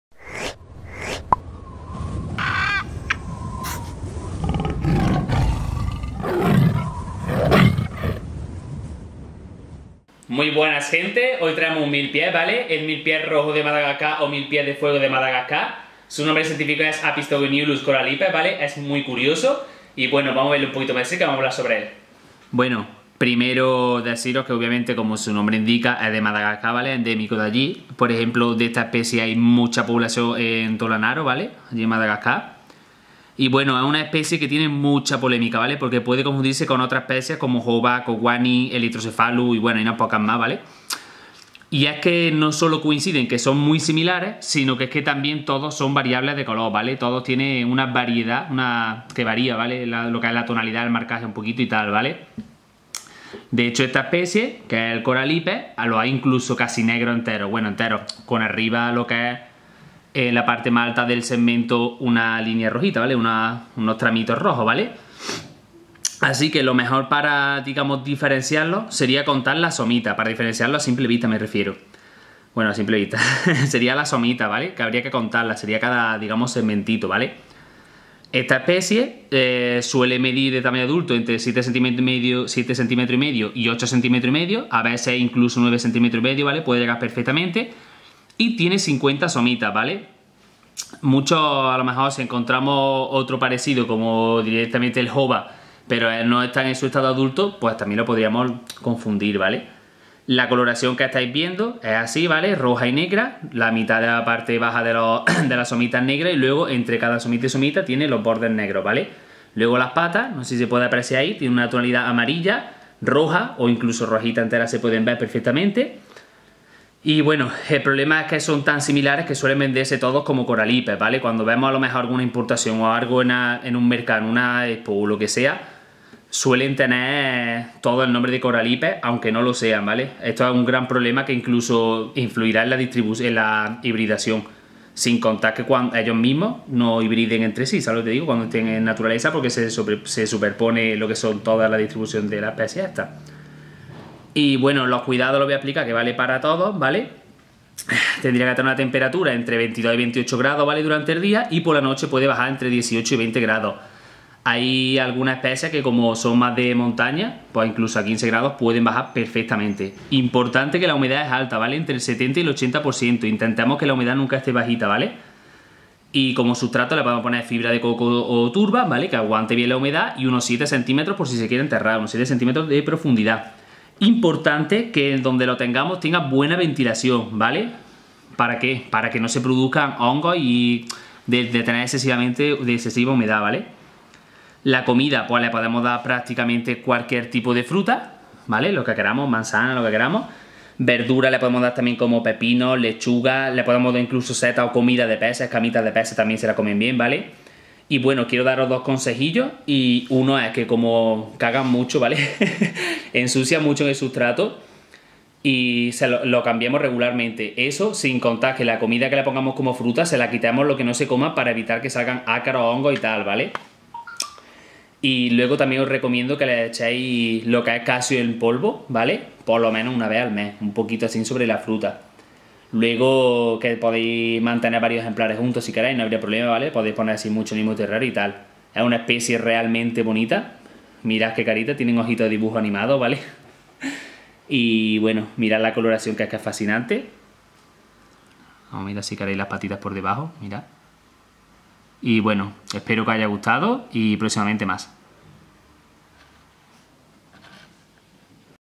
🐛 Milpiés de fuego de Madagascar (Aphistogoniulus corallipes) - El asignado para el mercado 🐛
Muy buenas gente. Hoy traemos un milpies muy bonito. El Milpiés de fuego de Madagascar (Aphistogoniulus corallipes). Hay muchos problemas de venta con esta especie siendo otras especies muy similares. Hablaremos un poco sobre sus cuidados, temperatura, alimentación ..etc Esperamos que os guste ^^  ↓ ↓ ↓

Hello people. Today we bring a very nice millipede. The Madagascan Fire Millipede (Aphistogoniulus corallipes). There are many problems of sale with this species being other species very similar. We will talk a little about his care sheet, temperature, feeding ..etc We hope you like ^^ Thanks

SABER ANIMAL